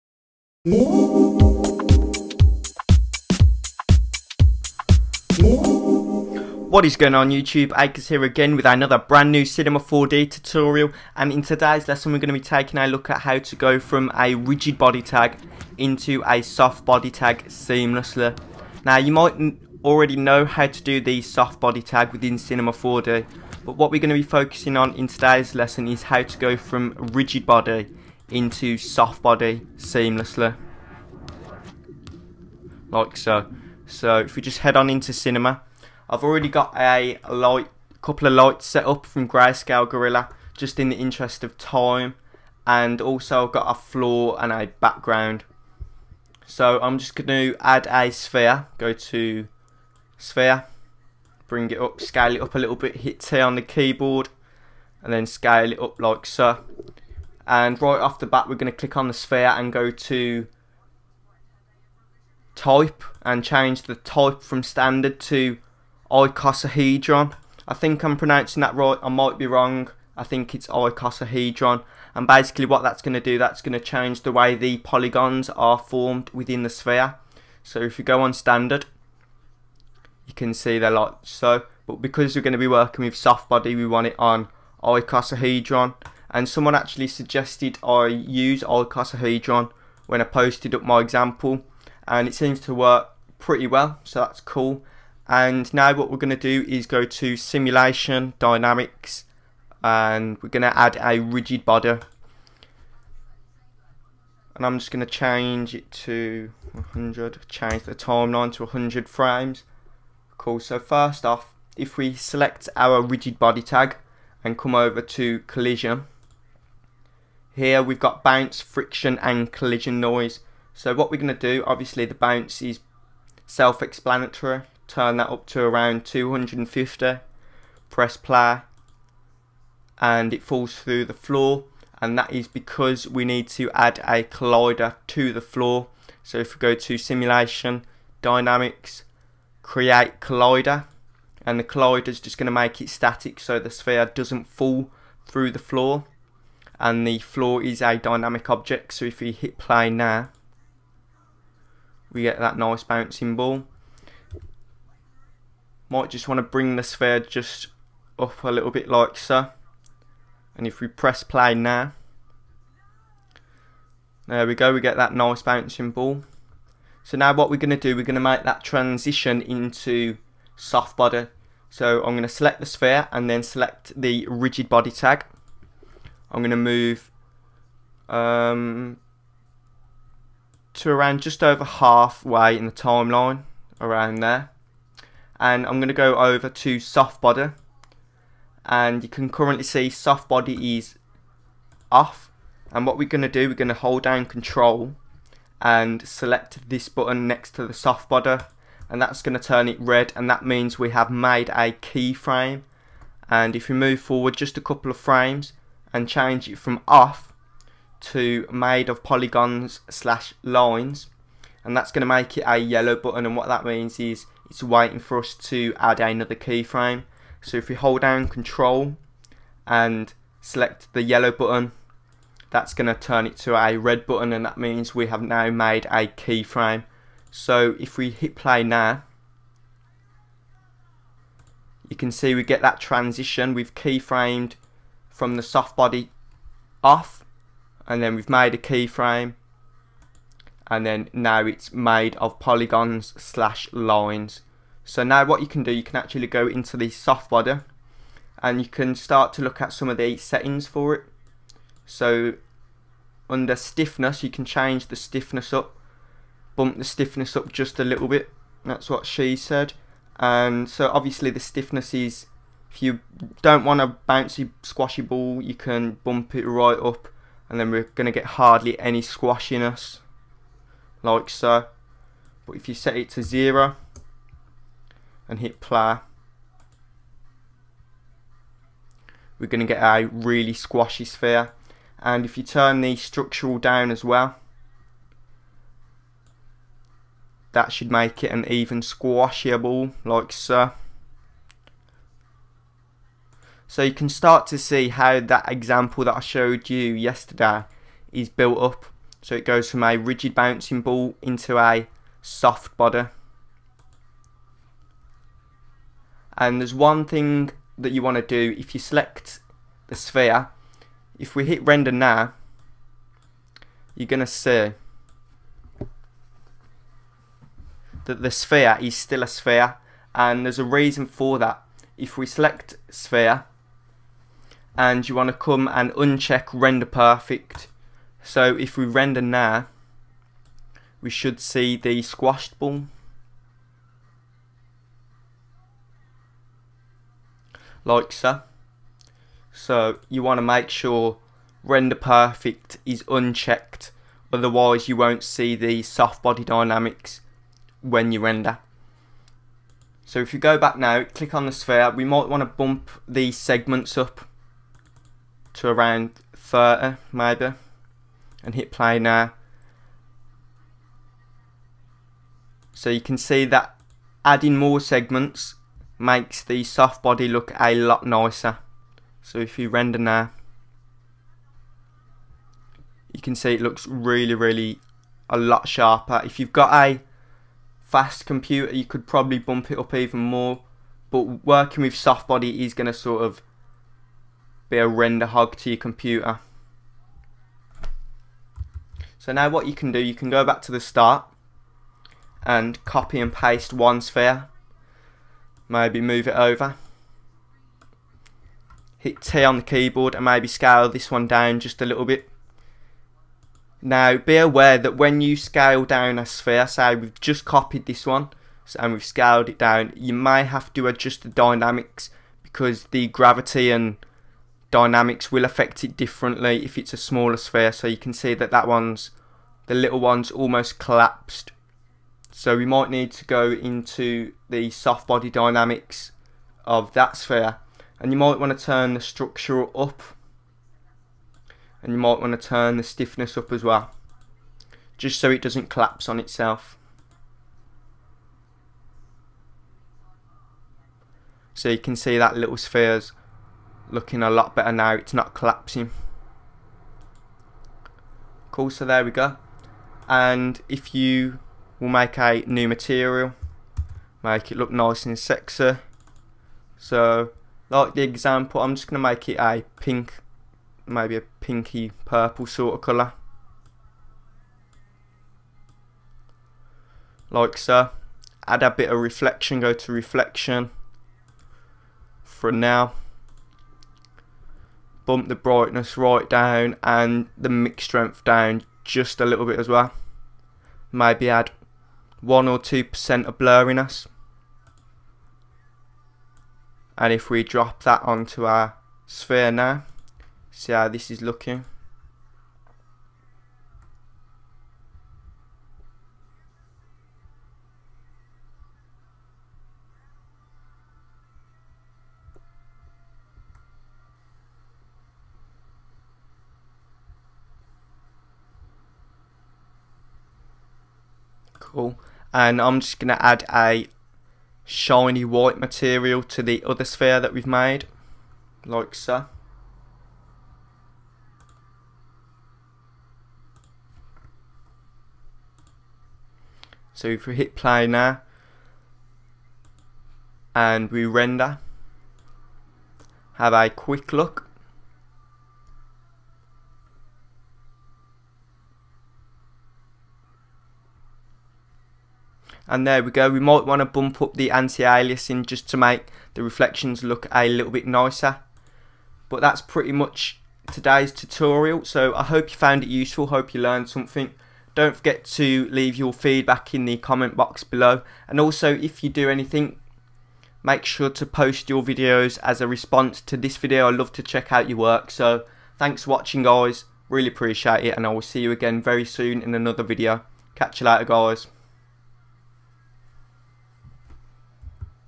Cinema 4D r12 Dynamics Tutorial: Rigid to Soft Body - AcrezHD
In this Cinema 4D r12 Dynamics Tutorial we look at how to go from a Rigid Body Dynamics tag into a Soft Body Dynamics tag seamlessly.

Dont forget to rate the video if you found it helpful, every 'Like' helps, thanks.